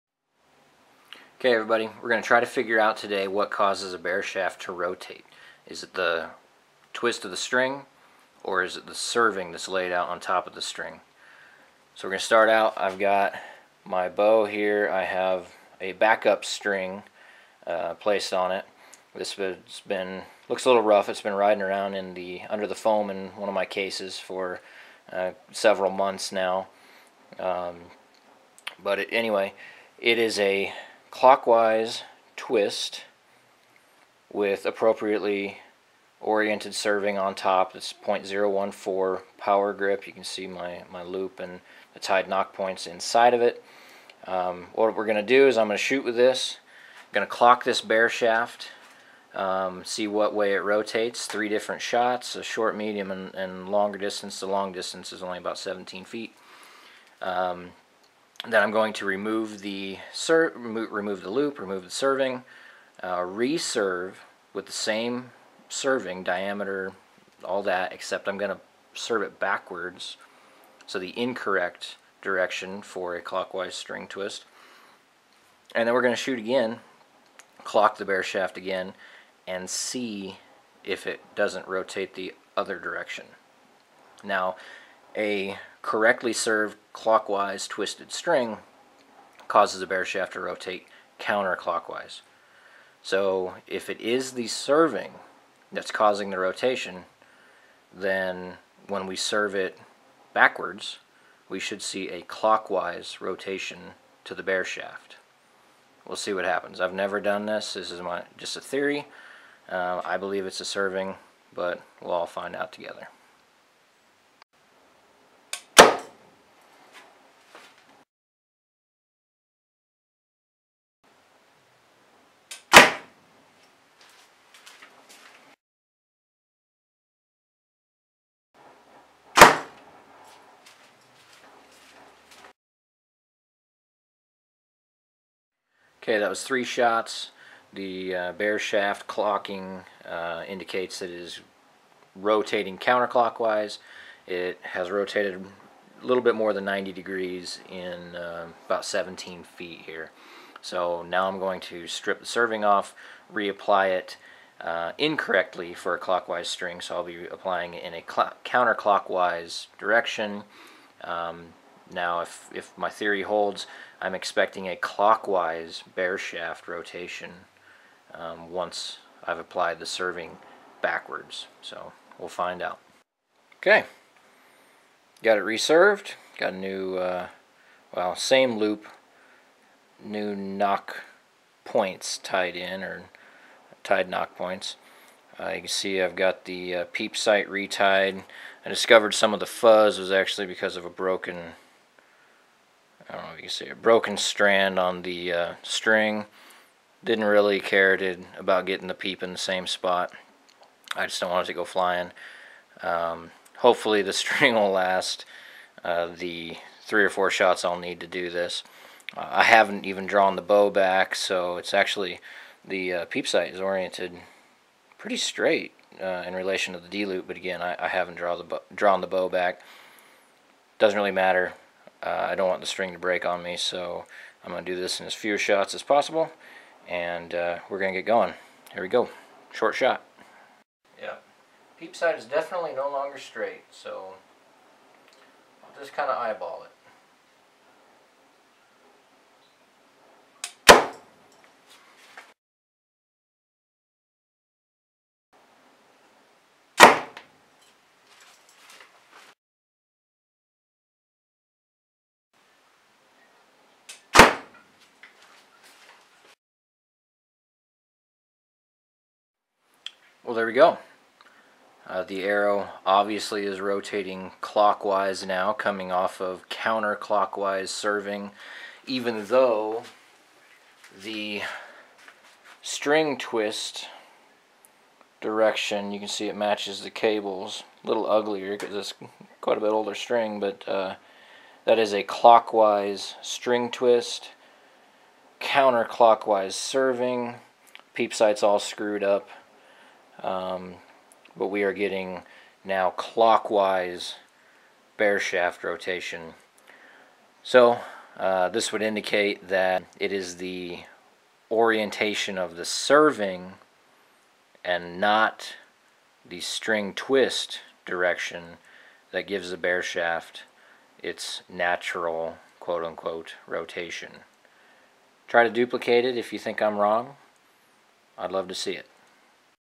What causes bareshaft rotation? String twist or serving orientation?
I screwed up a backup string I had to find out what makes a bareshaft rotate without fletching.